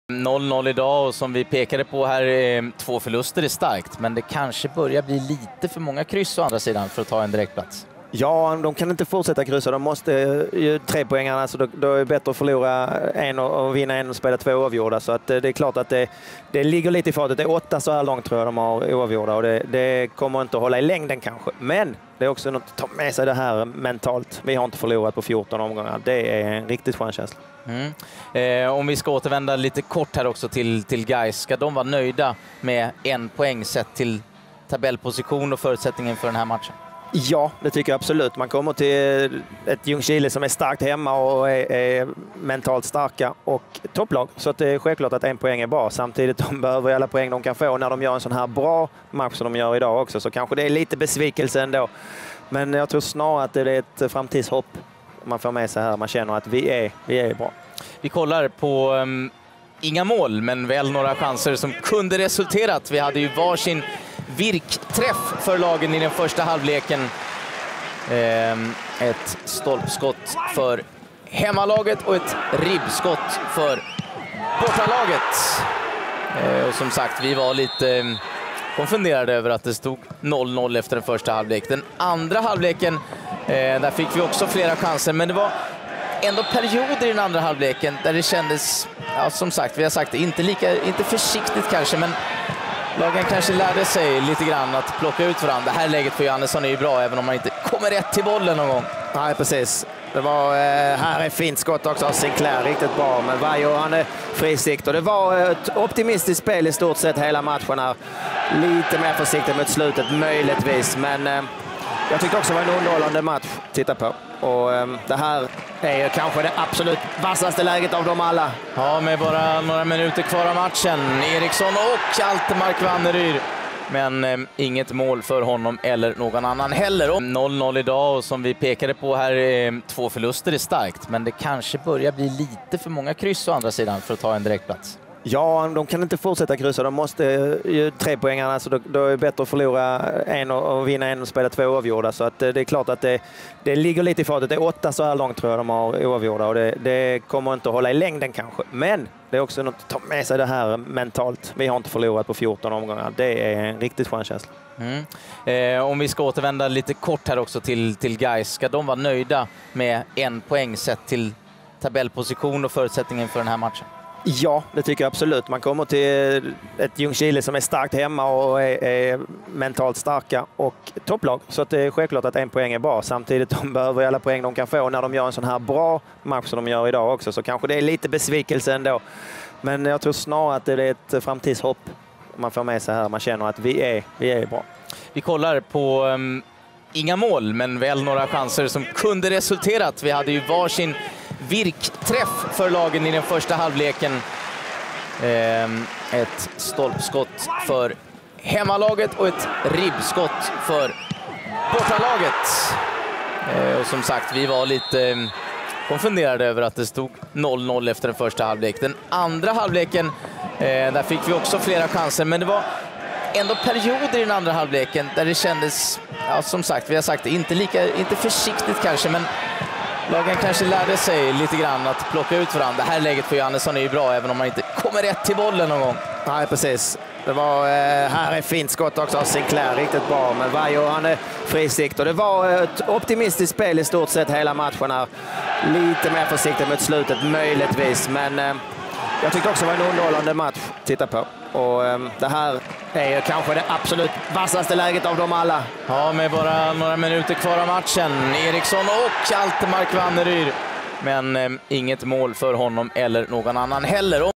Superettan: "Gais ska vara nöjda med 0-0" - TV4 Sport
TV4 Sport från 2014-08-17: Se höjdpunkterna från Ljungskile-Gais